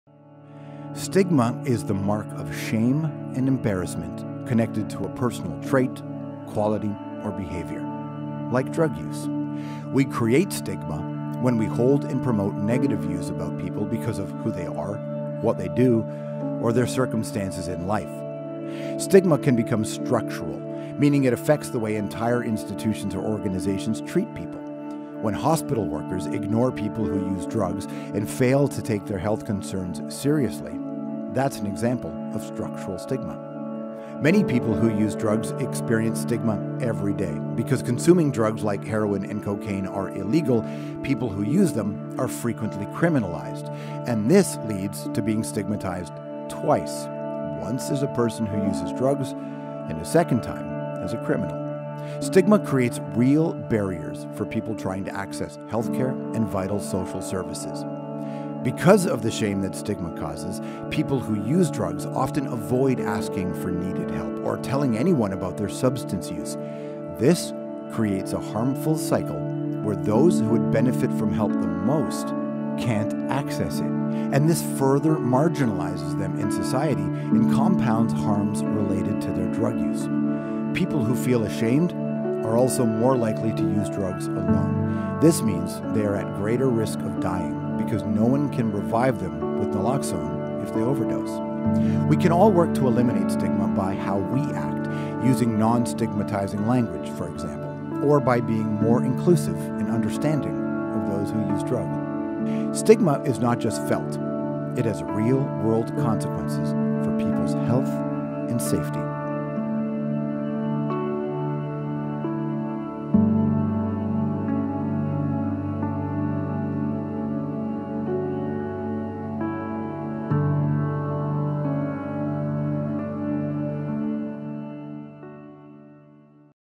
Stigma and Substance Use
At the heart of our broken system of drug policies lie underlying assumptions about people who use drugs. These negative stereotypes create laws that prohibit drug use and influence how we treat people who use substances. Stigma is both individual and systemic—how we treat others, and how institutions like hospitals and the legal system treat people. These negative and inaccurate stereotypes of people who use drugs have been borne out in popular culture, media, and everyday interactions. They create barriers to accessing social and health services as people who feel stigmatized will be less likely to seek help and talk openly about their substance use. By changing the way we think, we can change the way we act.